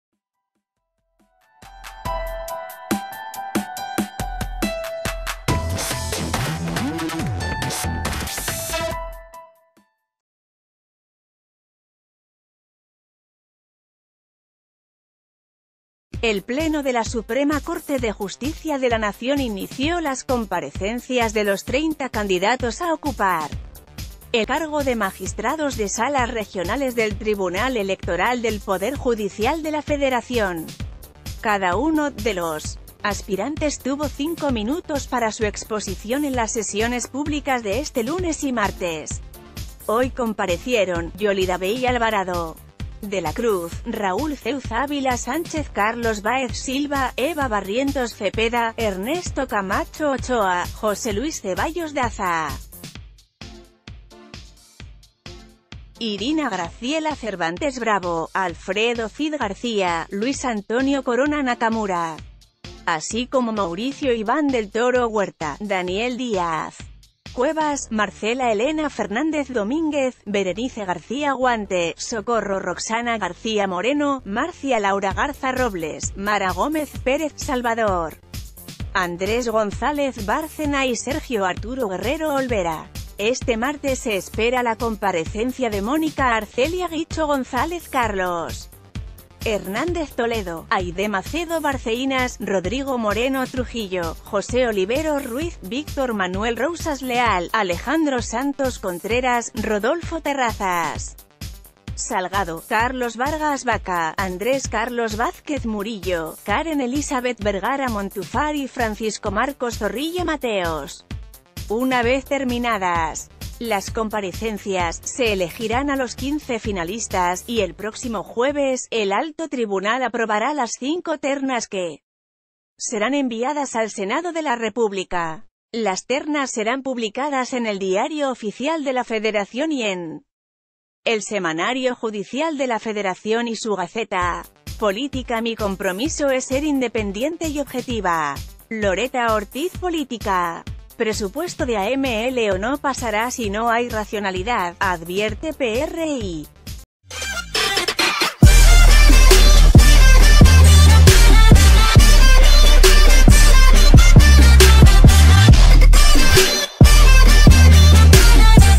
Inicia Corte comparecencias de candidatos a magistrados del TEPJF
El pleno de la Suprema Corte de Justicia de la Nación inició las comparecencias de los 30 candidatos a ocupar el cargo de Magistrados de Salas Regionales del Tribunal Electoral del Poder Judicial de la Federación. Cada uno de los aspirantes tuvo 5 minutos para su exposición en las sesiones públicas ...